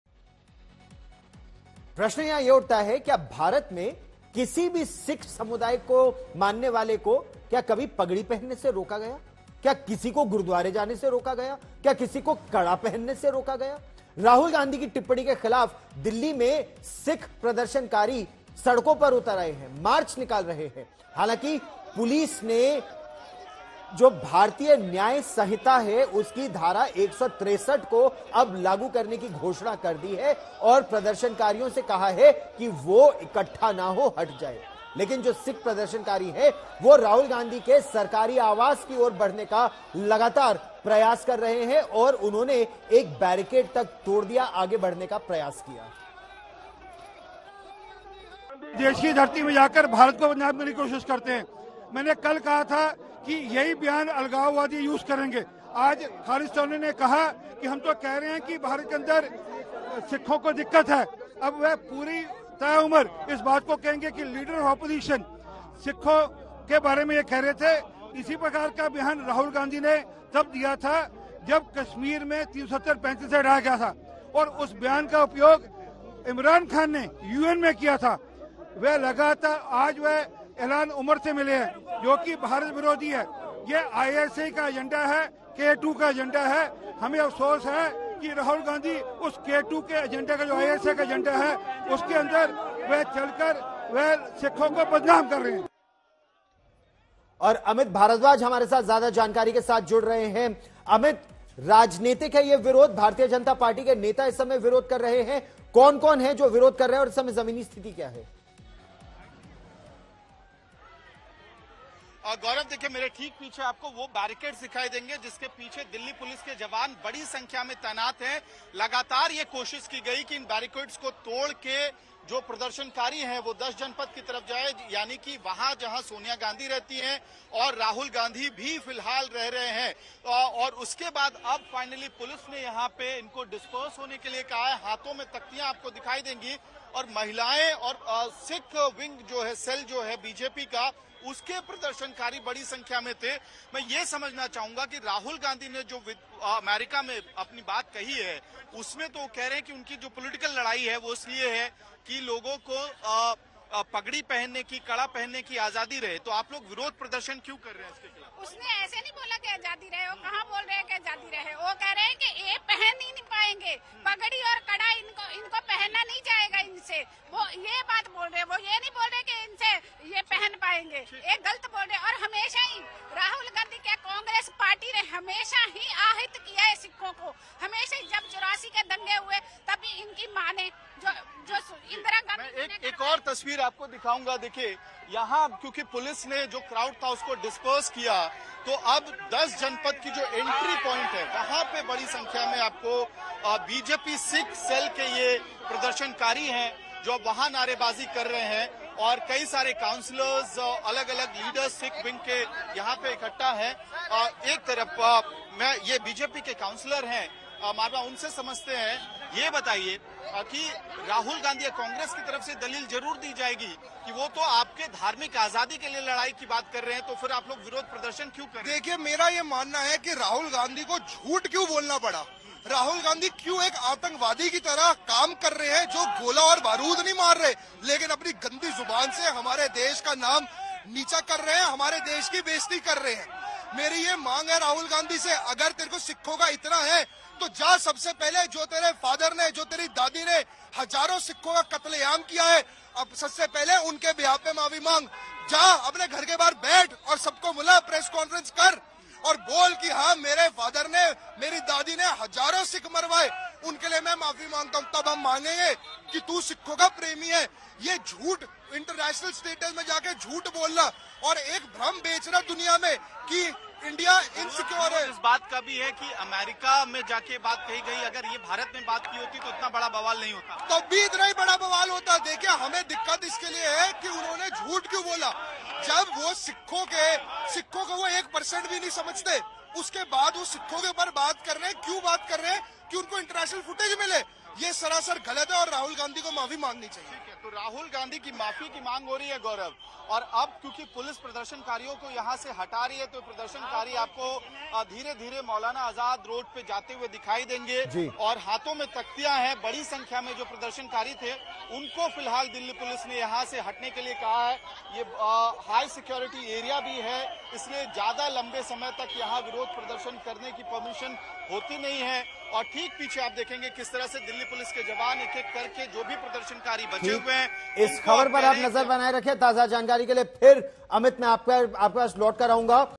Rahul Gandhi US Visit: Rahul Gandhi के सिखों पर बयान पर तनातनी | Rahul Gandhi On Sikhs | BJP
राहुल ने वर्जीनिया में कहा है कि वो जो लड़ाई लड़ रहे हैं वो राजनीतिक नहीं है, बल्कि इस बारे में है कि क्या सिखों को भारत में पगड़ी पहनने की इजाजत दी जाए? उनकी लड़ाई सिर्फ सिखों के लिये नहीं बल्कि सभी धर्मों के लिये है। उधर खालिस्तानी आतंकी गुरपतवंत सिंह पन्नू ने राहुल गांधी के बयान का फायदा उठाते हुए कहा है कि उन्होंने बोल्ड स्टेटमेंट दिया है, पन्नू के बयान के बाद बीजेपी ने राहुल पर और भी जोरदार हमला बोला है. Rahul Gandhi के सिखों के अधिकारों पर बयान ने राजनीतिक हलचल मचा दी है. भारतीय जनता पार्टी ने इस पर कड़ी नाराजगी जताई है.